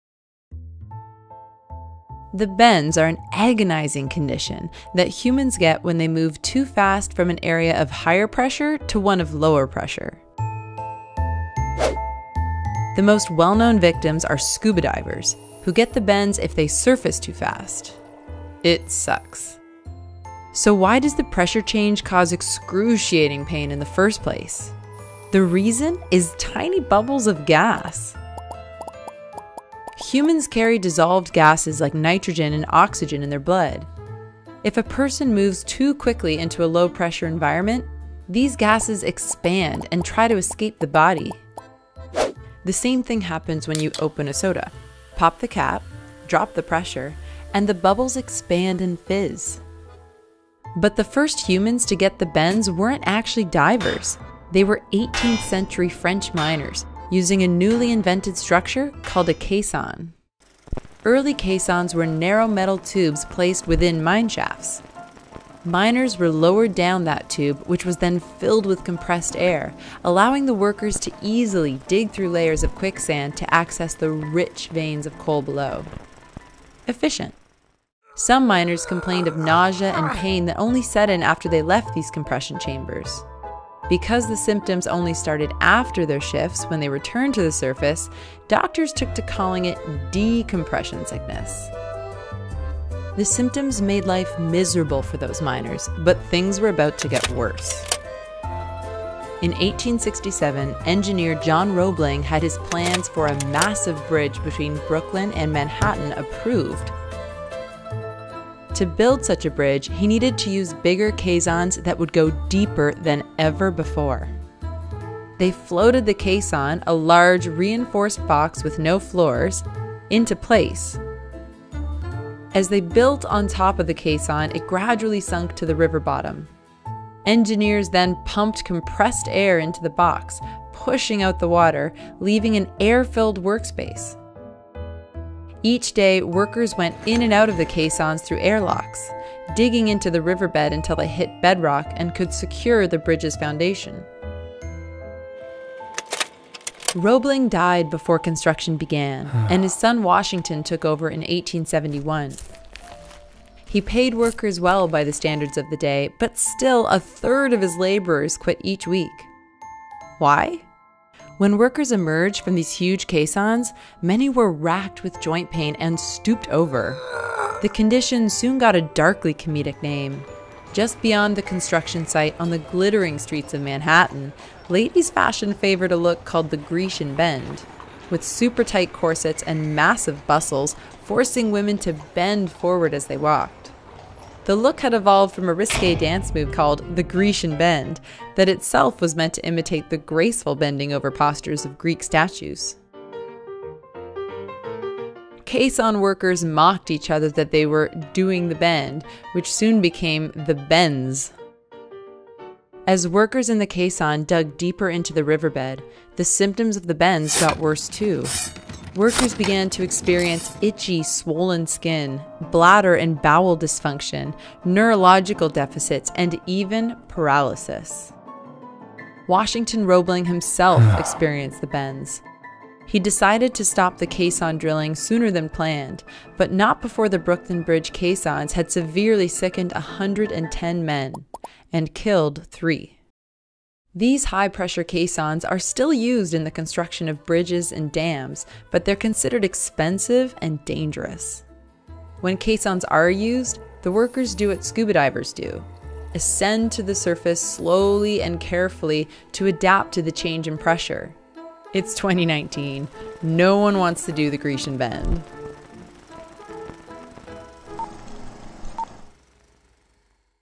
A High-Pressure Job
“The bends” is a condition rightly feared by SCUBA divers. Also known as decompression sickness, it’s triggered when a diver surfaces too quickly after spending time diving at depth. Decompression does weird things to the human body, and the pain can make people bend and contort themselves—hence the unusual name. But there’s another reason why some of the first sufferers used the term the bends to describe their then-mysterious affliction, and it had to do with fashion trends in glitzy glamorous 1860s Manhattan.

No, folks weren’t SCUBA diving in the 1860s. Though the first bends sufferers were technically working under the sea, they were actually construction workers. In this video, we dig deep into the history of this well-known diving risk, what it actually is, and how it got its name.

If you enjoyed this video, you’ll find lots of related stories on our You can sign up for our free weekly newsletter

Edited and animated by Katrina Pyne
Written and researched by Amorina Kingdon
Narrated by Katrina Pyne

Provided media:
Custom can illustration by Mark Garrison
Lower Manhattan skyline and East River, viewed from Brooklyn circa 1908 by Geo. P. Hall & Son, courtesy of the New-York Historical Society
Bull kelp illustration by Pina Clothing